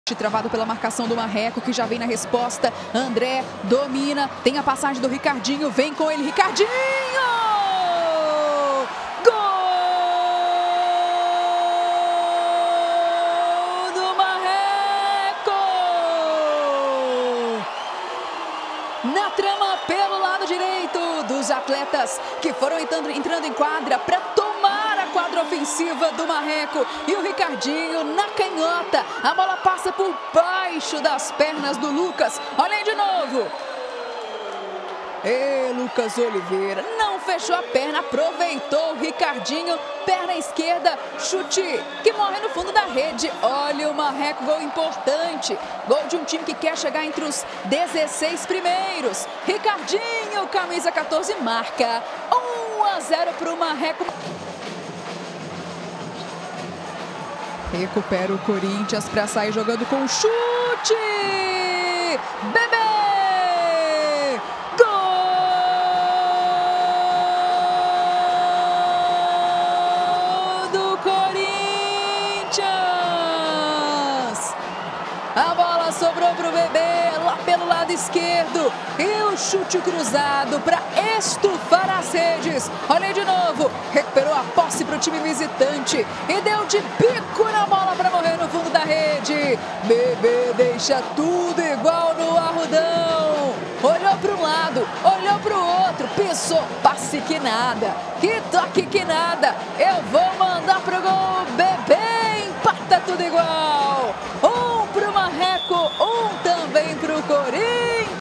LNF MRJACK.BET 2023 - 13ª RODADA - GOLS - MAR 1 X 1 COR
GOLS:
MARRECO - RICARDINHO
CORINTHIANS - BEBÊ
TRANSMISSÃO: SPORTV